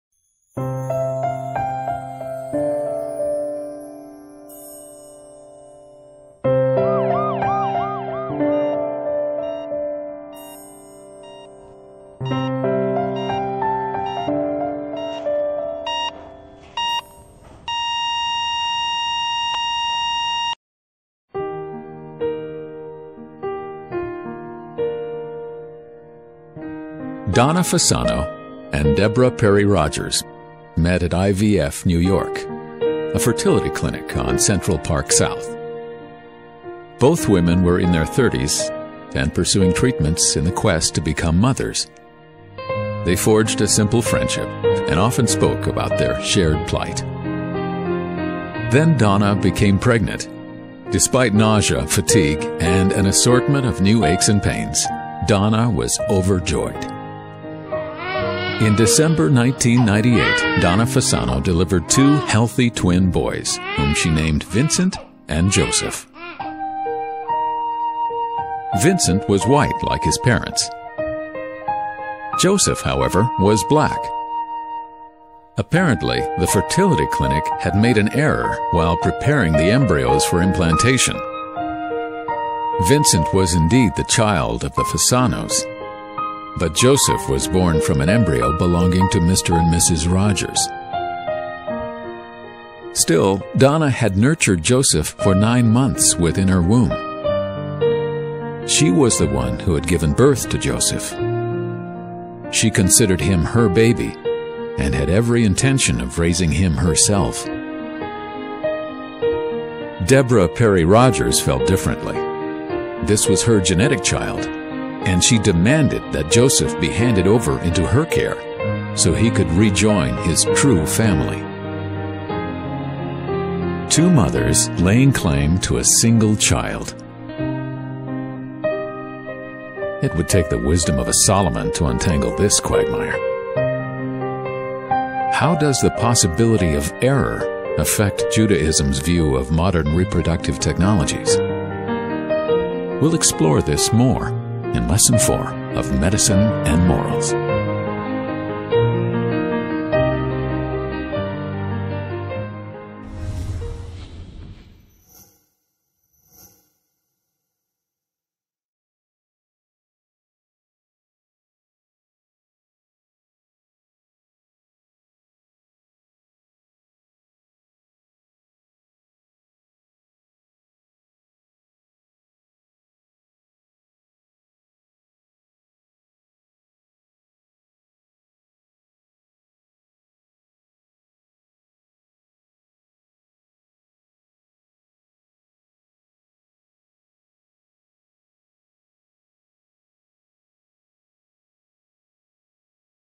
Medizin & Moral - Lektion 4 - Eltern werden: die Ethik bei der Reproduktionsmedizin
Direkt von den heutigen Schlagzeilen, behandelt diese Kurseinheit die Entscheidungen mit denen Paare konfrontiert werden. Von Designer-Babys zu der Ei- und Samenspende, von der Vermeidung genetisch bedingter Krankheiten, zu Leihmüttern -- komplexe Fragen werden offen besprochen. Lebhafte Diskussionen sind garantiert.